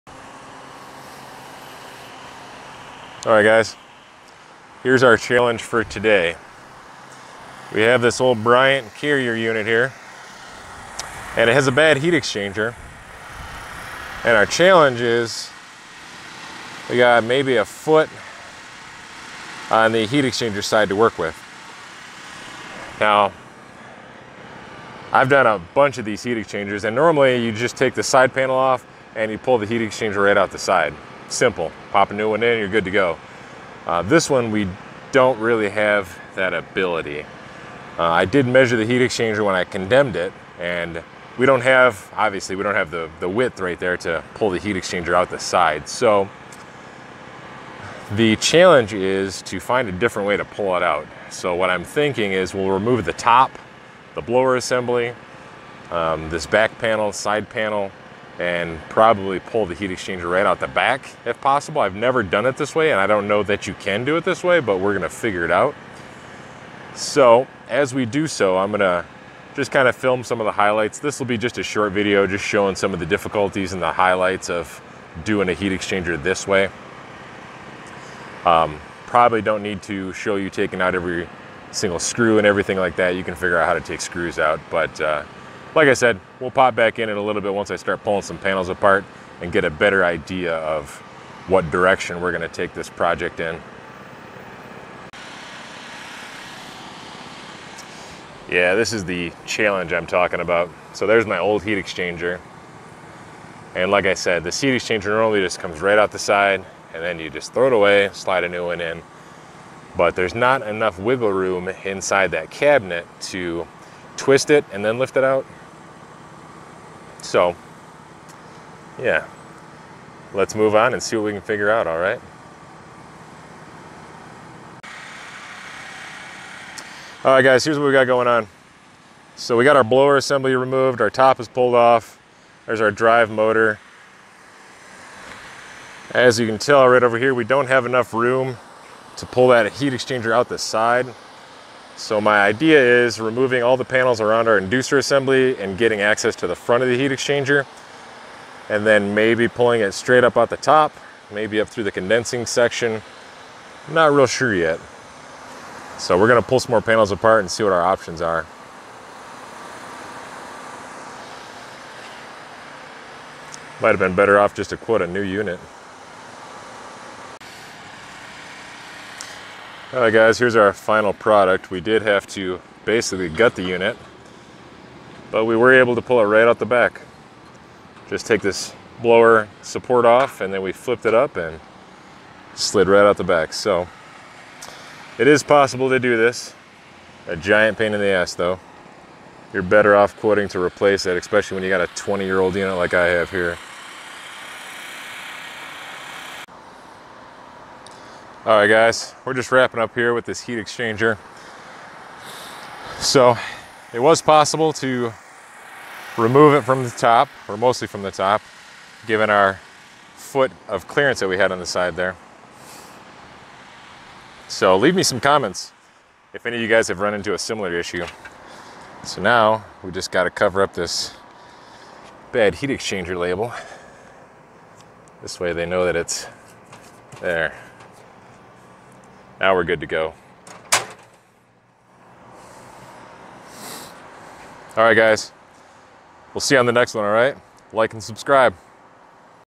Replacing a heat exchanger in a Bryant package unit
Today in this short video we have a rooftop unit positioned very close to a rooftop wall that is in need of a new heat exchanger. I go through a few highlights and show how I handled this project. Enjoy